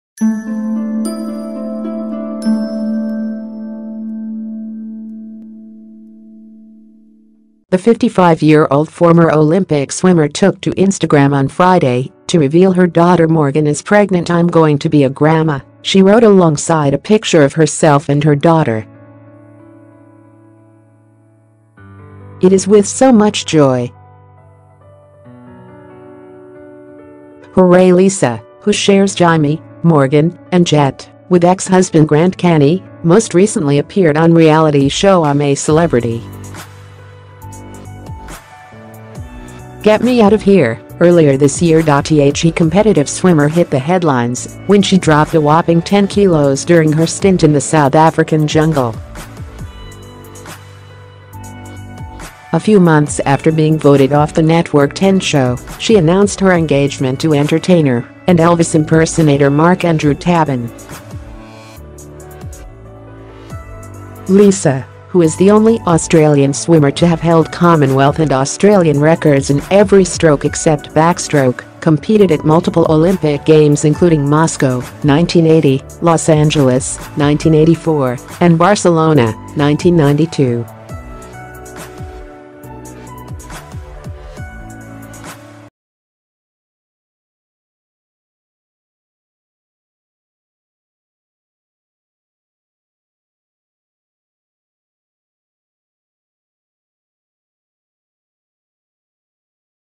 Former Olympic swimmer Lisa Curry reveals she's going to be a grandmother
Lisa Curry has some news!

The 55-year-old former Olympic swimmer took to Instagram on Friday to reveal her daughter Morgan is pregnant!

"I'm going to be a GRANDMA!" she wrote alongside a picture of herself and her daughter. "It is with so much JOY & EXCITEMENT to announce that our beautiful daughter Morgan and hubby Ryan are having a BABY!!"

She added to her 22,000 followers, "This kid is going to be spoilt rotten. Hooray!"